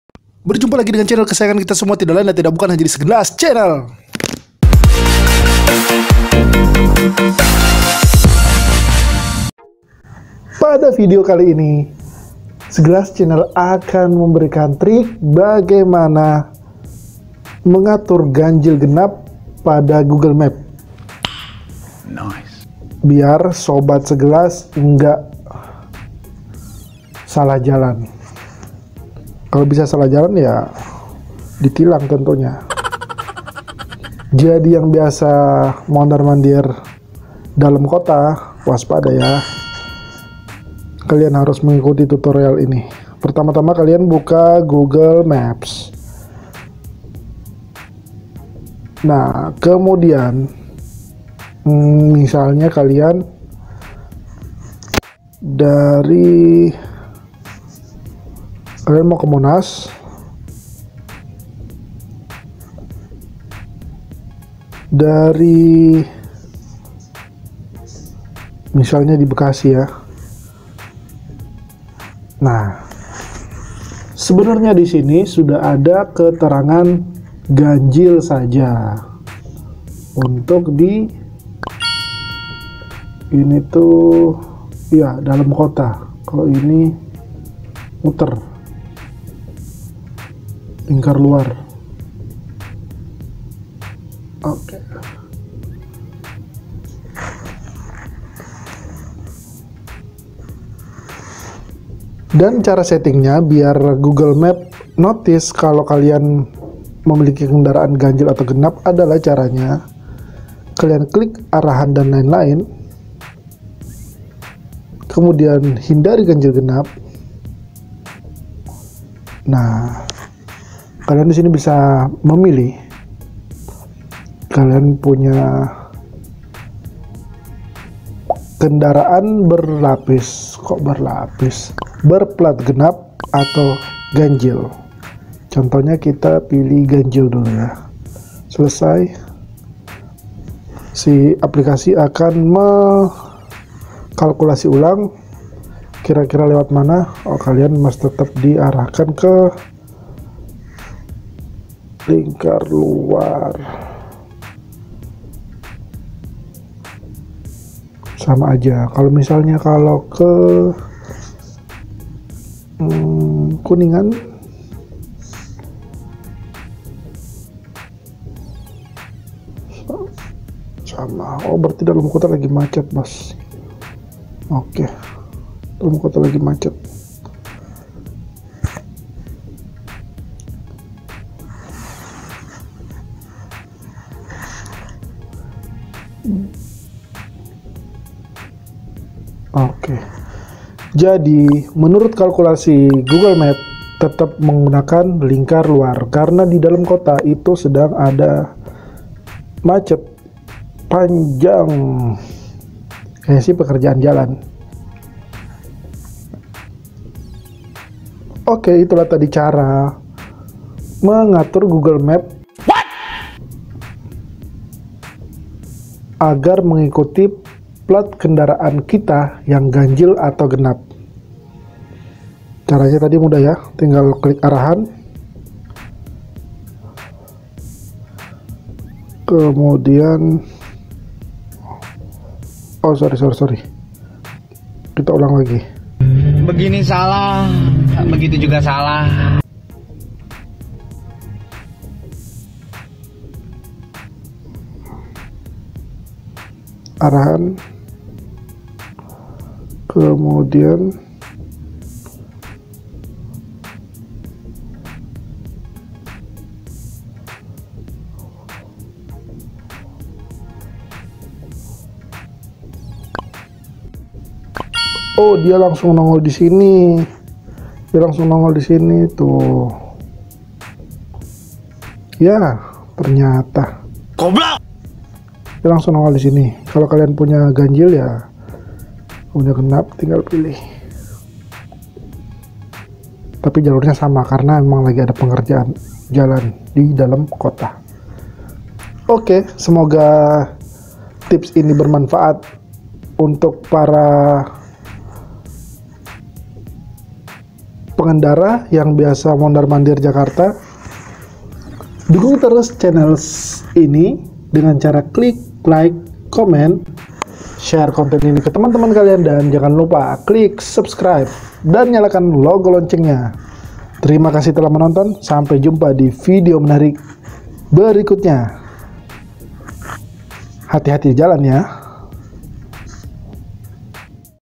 Cara Mengatur Jalur Ganjil Genap di Google Maps Biar Gak ditilang
Buat sobat segelas yang ingin pergi ke jakarta bingung mau pilih jalur ganjil genap nih cara ampuh biar tidak nyasar dan gak di tilang

cheeersss...